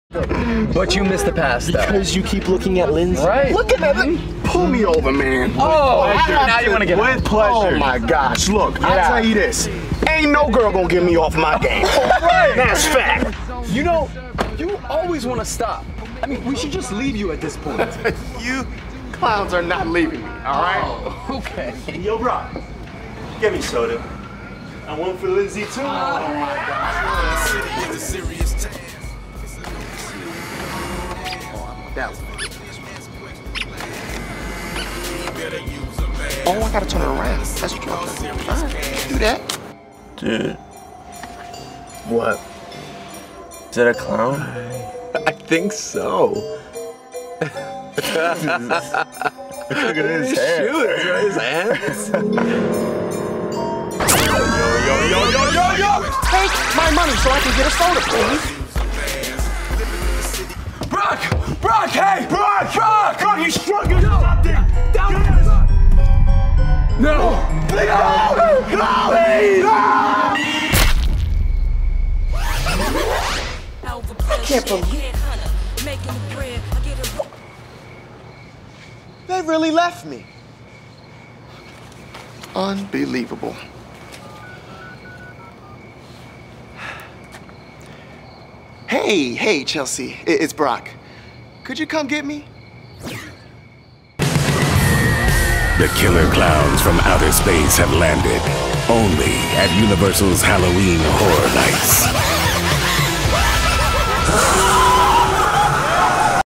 Killer Klowns From Outer Space – Halloween Horror Nights 2022
The Killer Klowns from Outer Space have landed. If you let them turn you into a bloody goo, then the joke’s really on you.